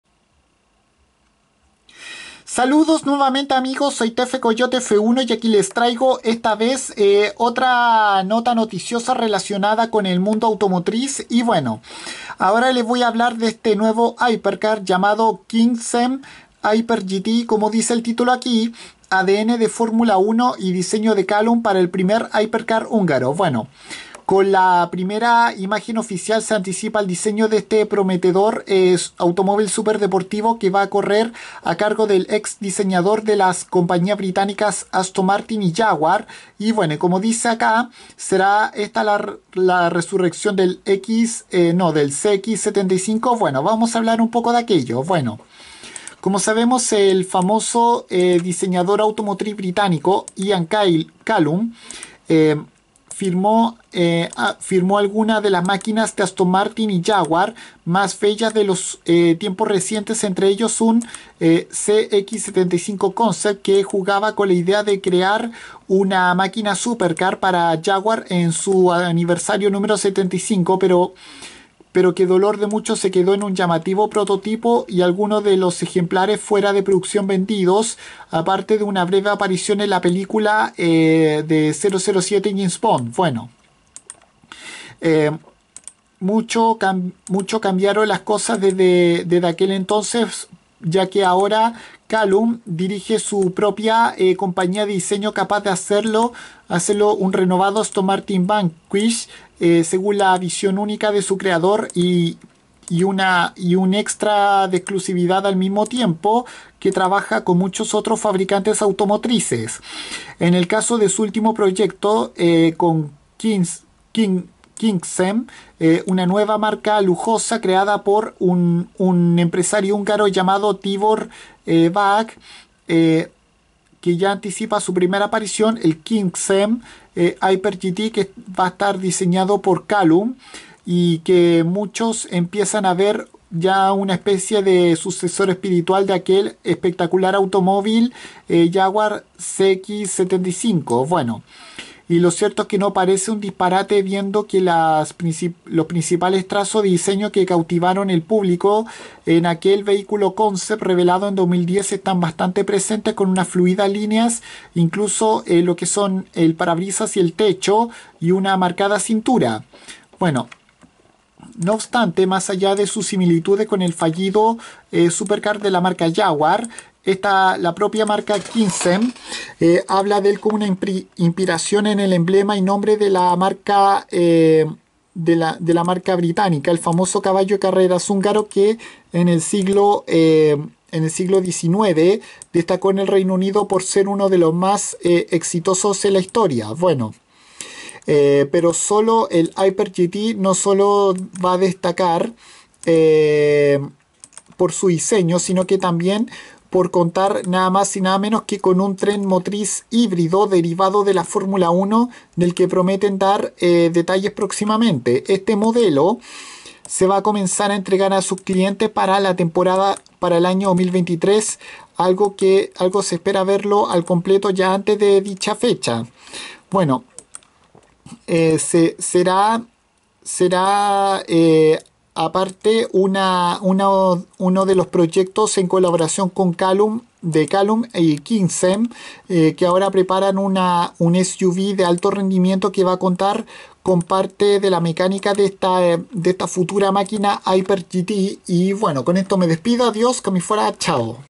Kincsem HyperGT información
En este vídeo les comento sobre como será el primer automóvil hypercar fabricado en Hungría que poseerá tecnología híbrida heredada de la Fórmula 1 que competirá contra los nuevos Aston Martin Valkyrie y Mercedes AMG Project One y que será el sucesor espiritual del fallido jaguar CX75 además será diseñado por Callum